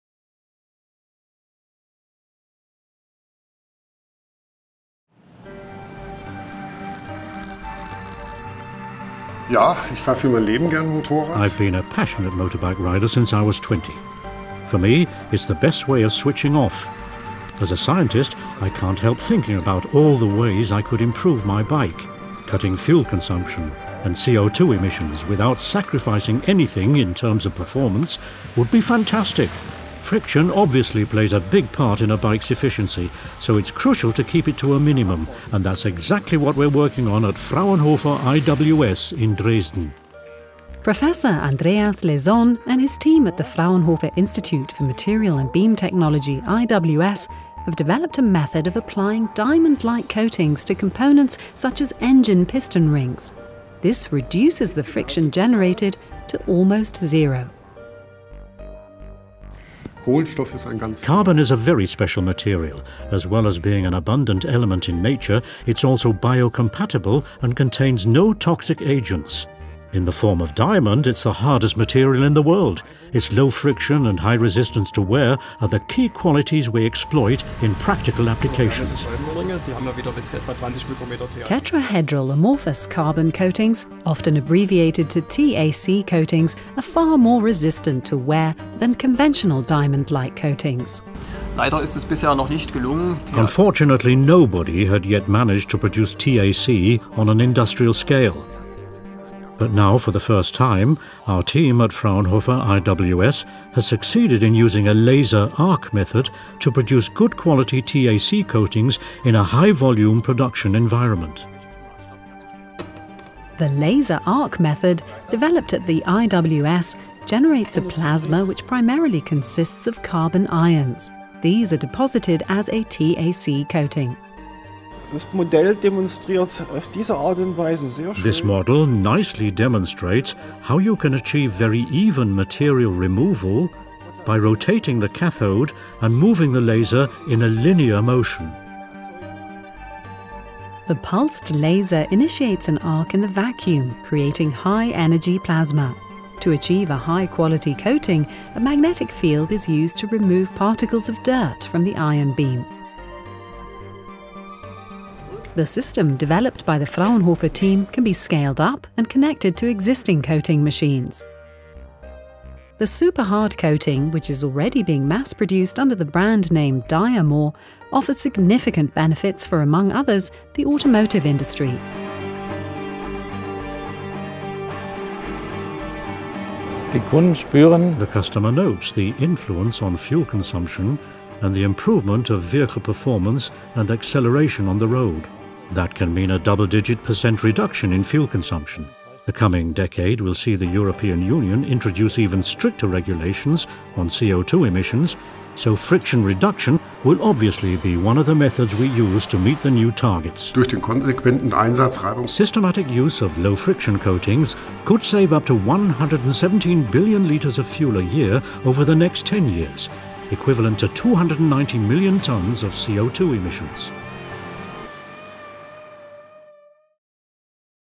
Diamond like coatings save fuel
Coating engine components with hard carbon reduces friction to almost zero – a development that could save billions of liters of fuel worldwide every year. Now researchers have developed a new laser method to apply the coating on the production line.

Coating engine components with hard carbon reduces friction to almost zero – a development that could save billions of liters of fuel worldwide every year. Now researchers have developed a new laser method to apply the coating on the production line. Scientists already know how to coat components with diamond-like carbon to minimize friction. But now  researchers of the Fraunhofer Institute for Material and Beam Technology IWS in Dresden have developed a laser arc method with which layers of carbon almost as hard as diamond can be applied on an industrial scale at high coating rates and with high thicknesses. By applying carbon coatings to engine components such as piston rings and pins, fuel consumption can be reduced.

Prof. Andreas Leson, Dr. Hans-Joachim Scheibe and Dr. Volker Weihnacht received the 2015 Joseph von Fraunhofer Prize for the development of the laser arc method and the application of ta-C coatings in volume production.